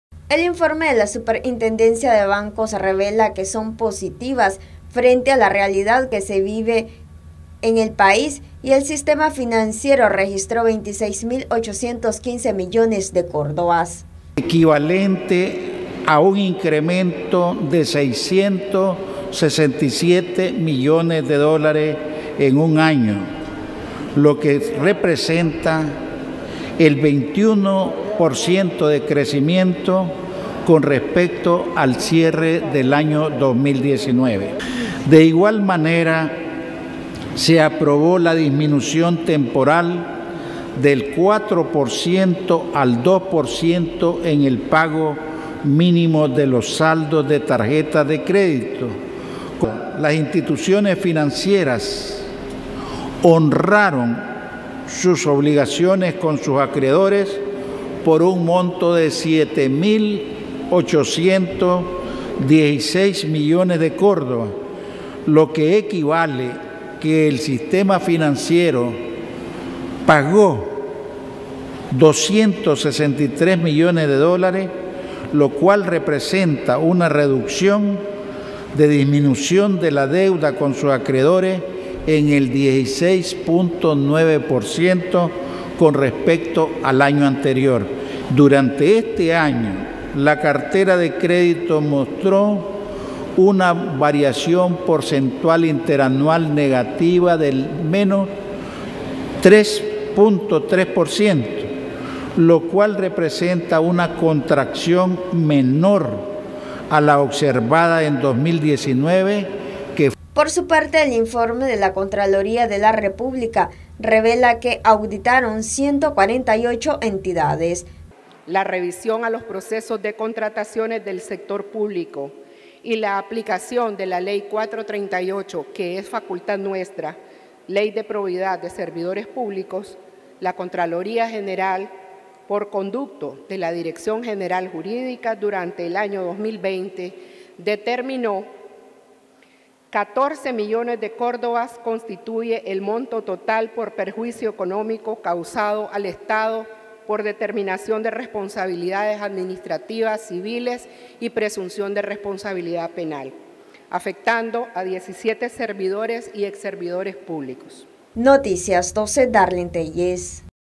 Contraloría y Superintendencia de Bancos presentan informe 2020 ante Asamblea Nacional
NosResistimosAlEmbargo Miembros de la Contraloría General de la República Y Superintendencia de Banco presentan informe 2020 ante el parlamento nacional en la que destacan algunos logros.